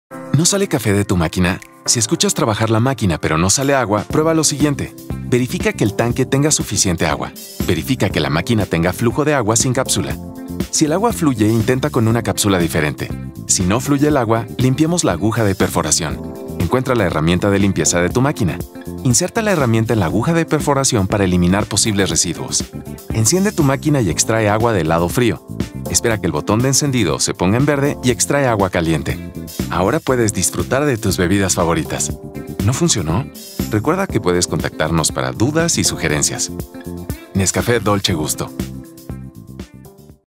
Máquina automática sin flujo
Si tu máquina no tiene flujo, sigue estos pasos para verificar que no tenga una obstrucción que impida la salida del agua.